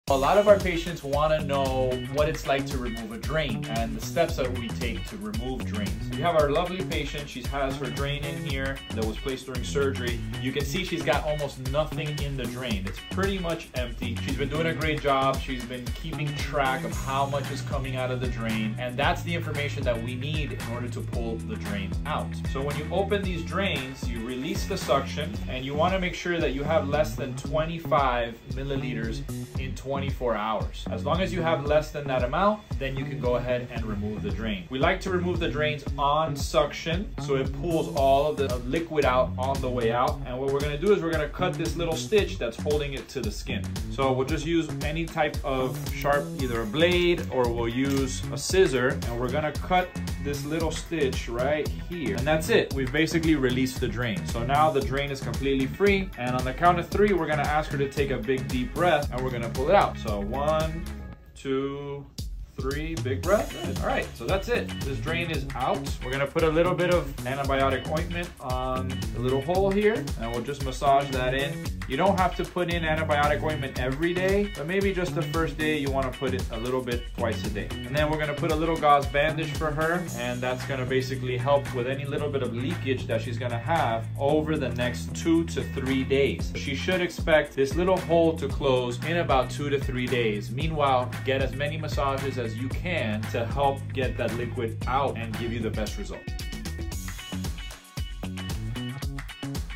How To Remove Drains with Dr. Zuri in Miami | Zuri Plastic Surgery
Drains are tubes placed near surgical incisions after specific procedures, like tummy tucks, to remove pus, blood or other fluid, preventing it from accumulating in the body. A lot of patients request to see what the process looks like to remove drains, so here’s a video walking you through it!